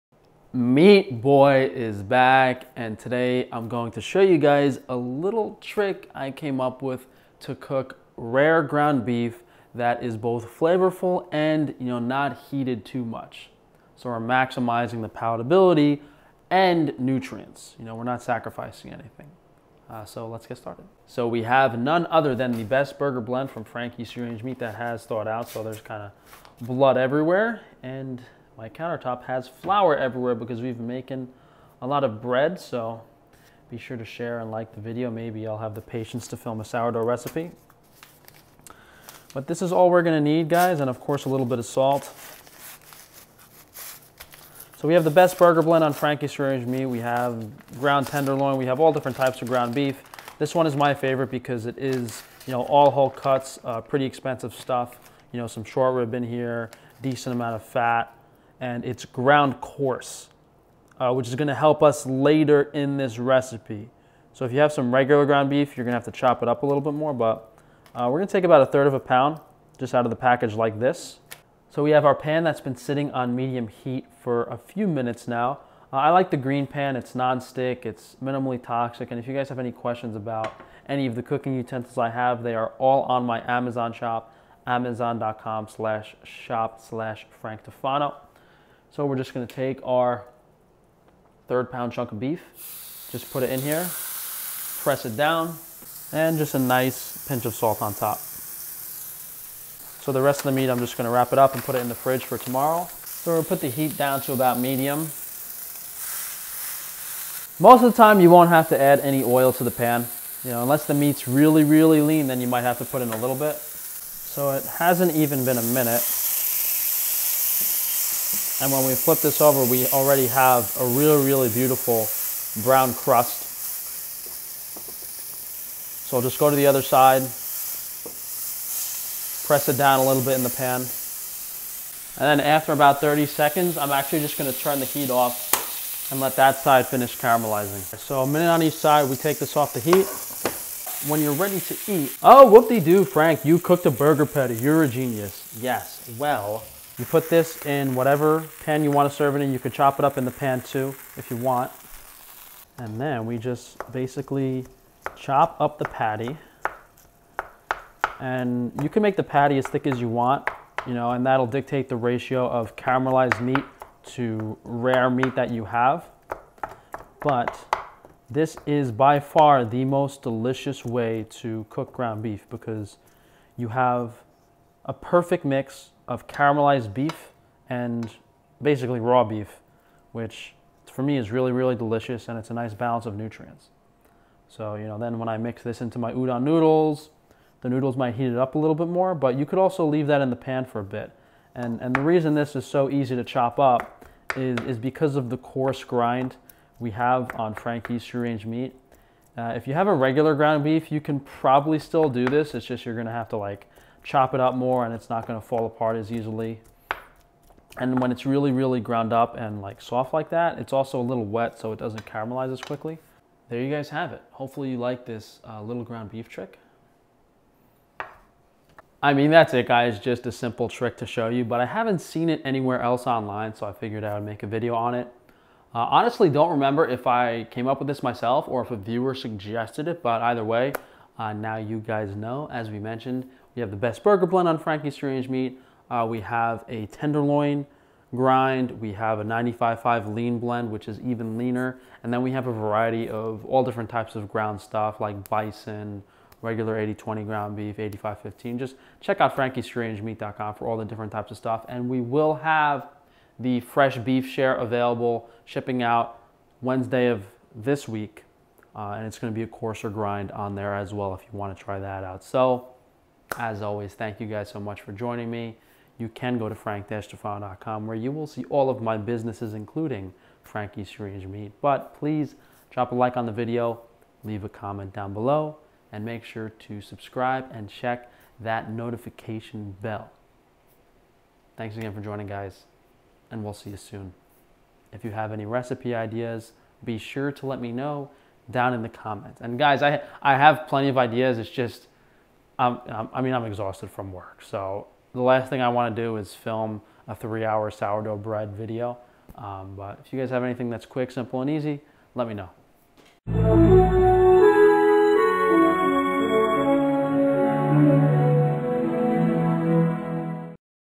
DELICIOUS "Rare" Ground Beef!
00:00: - MEAT BOY is back!
00:20 - Ground beef
01:11 - Green Pan
01:35 - Cooking the "Burger"
02:22 - Chopping
03:38 - Thanks for joining!